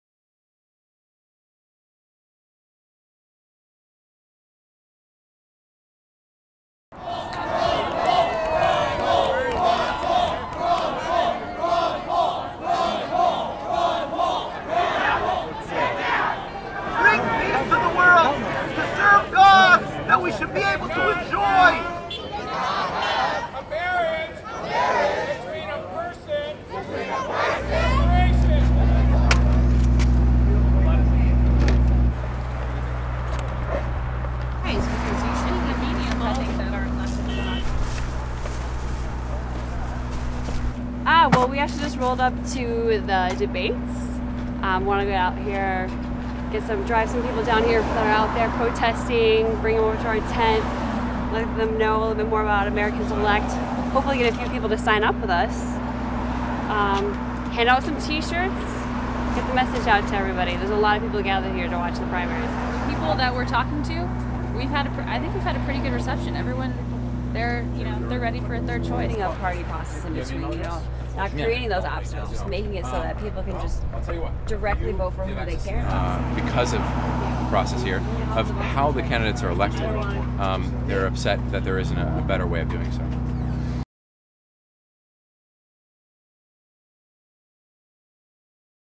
Americans Elect Outside The GOP NBC / Facebook Debate
Amidst protests the Americans Elect team hits the streets! Kerry Hanley, Dagny Leonard and Kameron Schleifer setup outside the Concord, NH GOP morning debate.